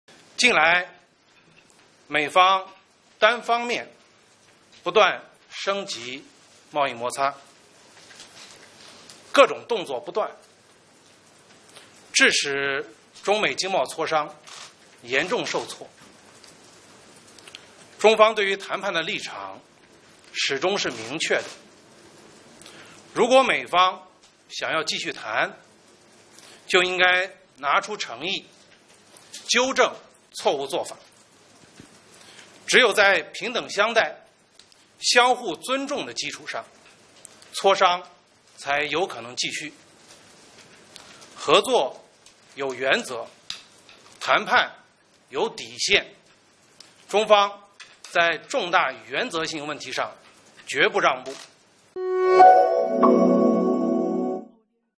中国商务部谈中美经贸磋商：在重大原则性问题上决不让步
5月23日，商务部在北京召开例行新闻发布会。商务部新闻发言人高峰表示，美方单方面不断升级贸易摩擦，致使中美经贸磋商严重受挫。只有在平等相待、相互尊重的基础上磋商才有可能继续。合作有原则，谈判有底线，中方在重大原则性问题上决不让步。